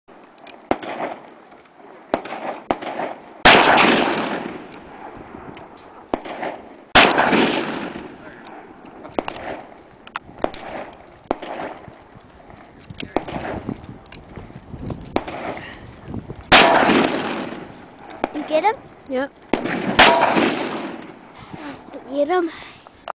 500 mag
crapy video but the only one I have of the gun that little girls like my brother in law cant handle